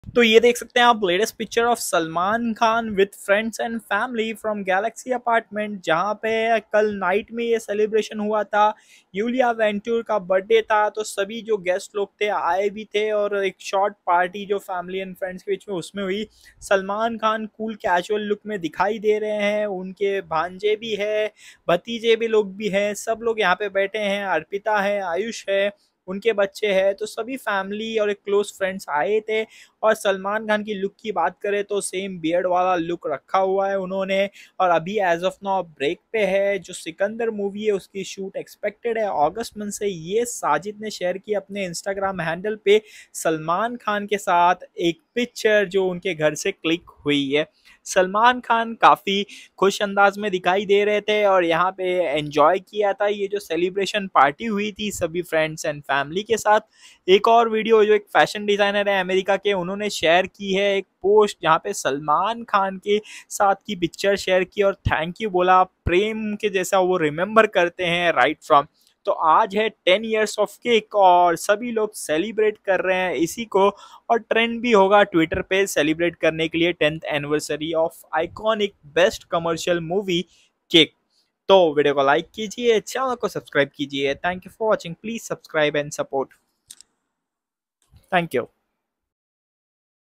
Latest Of 𝗦𝗮𝗹𝗺𝗮𝗻 Khan With Friends & Family From Iulia’s Birthday Celebration At Galaxy Apartment
Latest Of 𝗦𝗮𝗹𝗺𝗮𝗻 Khan With Friends & Family From Iulia’s Birthday Celebration